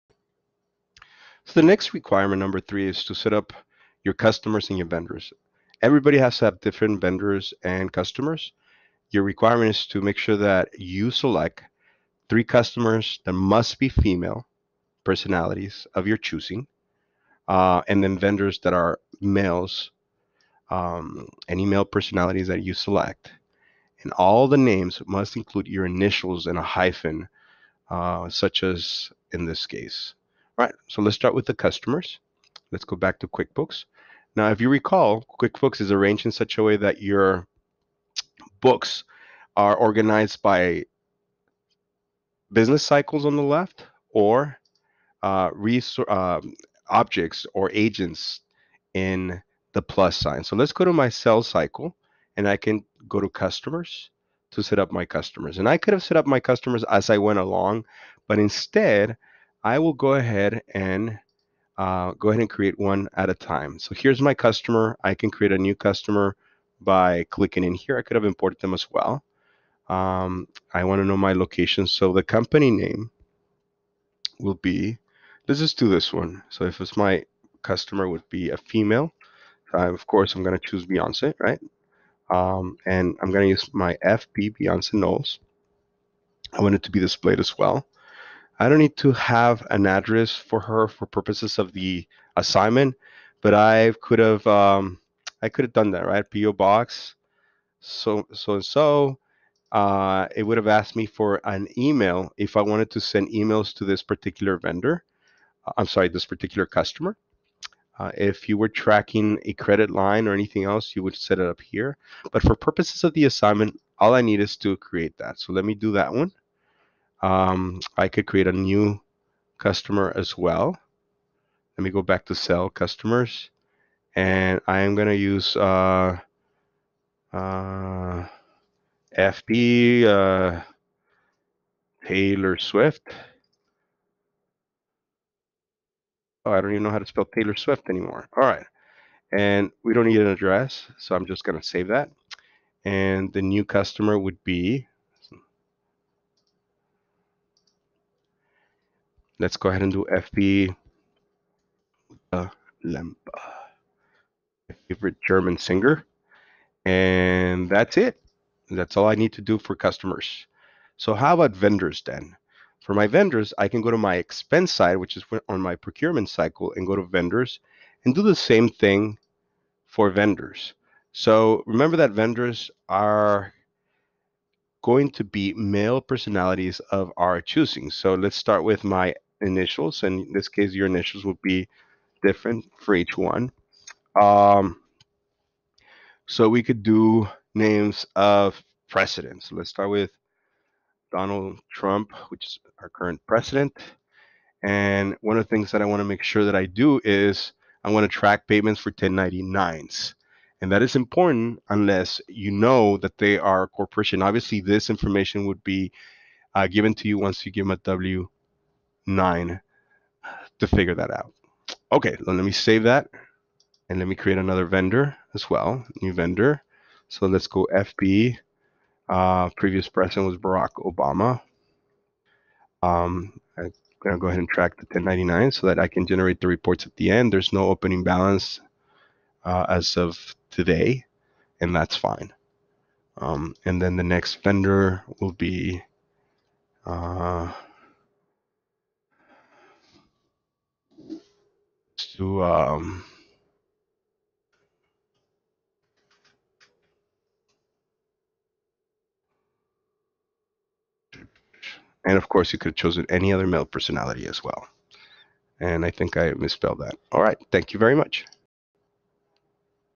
ACCT146-SP-Quickbooks-03-How to Setup Customers and Vendors
Accounting Information Systems
How to Setup Customers and Vendors in Quickbooks

Fernando Parra, CISA, Ph.D.
Fresno State University